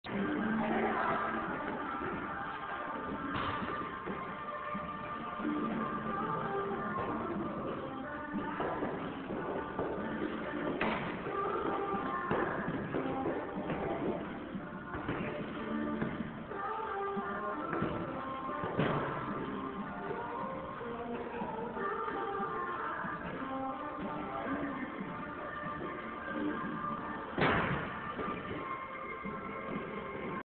Break dancing in the weight room
matt breakdances in the weight room